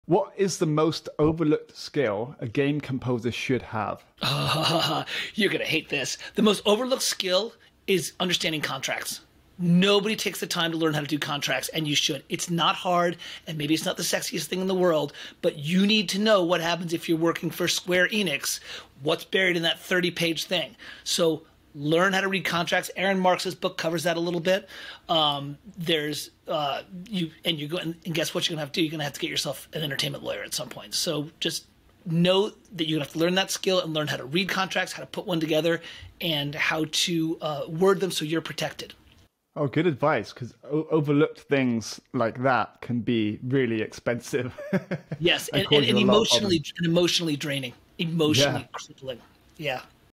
Mike Worth: The most overlooked skill a composer should have
What's the most overlooked skill a composer should have?